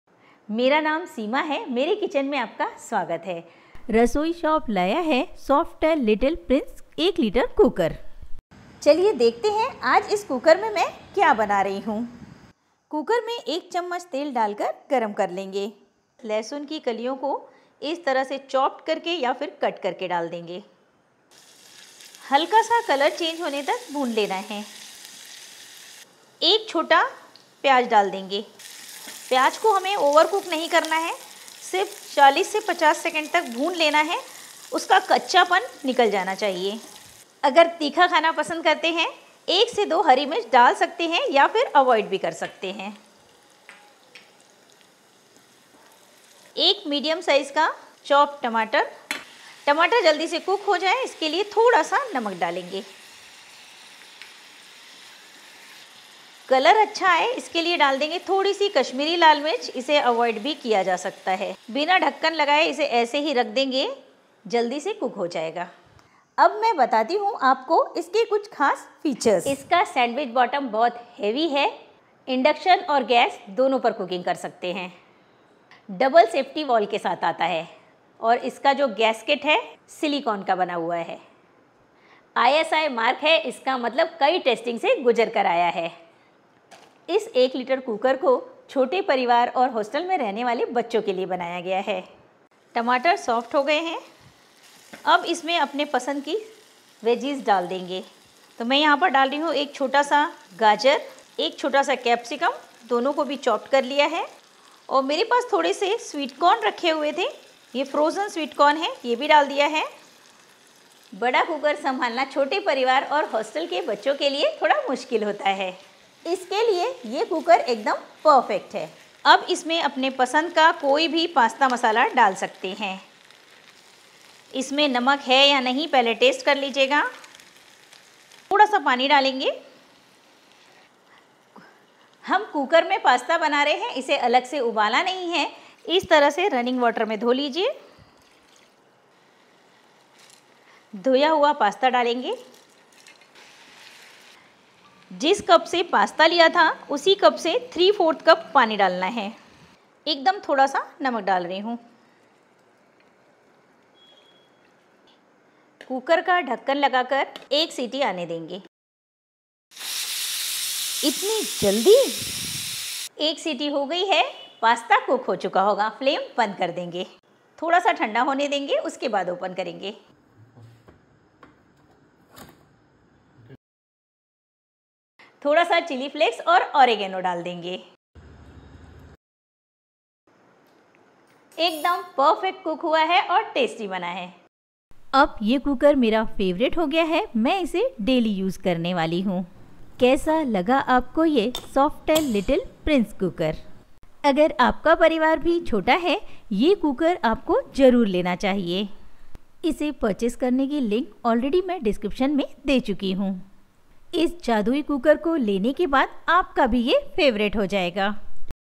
मैंने आज तक इतना टेस्टी पास्ता कभी नहीं खाया | Pasta in Softel Cooker | Seema's Smart Kitchen
इतनी आसान है कि मेरा बेटा बना लेता है .सबसे आसान रेसिपी बच्चे भी बना लेंगे...मैंने आज तक इतना टेस्टी पास्ता कभी नहीं खाया Without Hard-work Yummy Pasta | Seema's Smart Kitchen

Link for Softel Little Prince Stainless Steel 1 Litre Cooker

Link for Tripod cum Selfie Stick
Links for all the products I use: